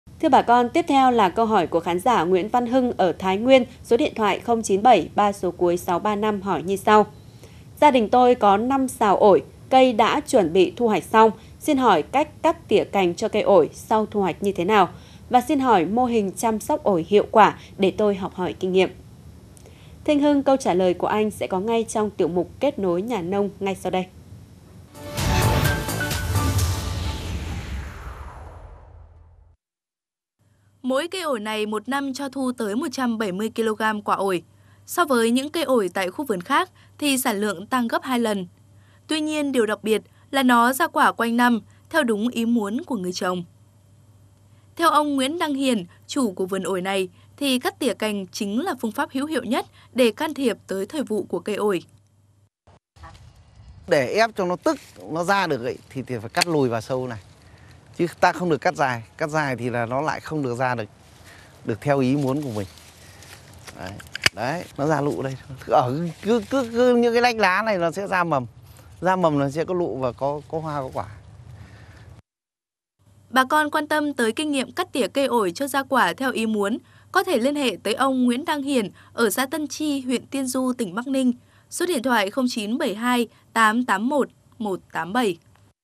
Kinh nghiệm cắt tỉa để cây ổi ra quả theo ý muốn I VTC16
VTC16 | Để cây ổi ra quả theo ý muốn, việc cắt tỉa cành giai đoạn sau thu hoạch đúng kỹ thuật đóng vai trò quan trọng.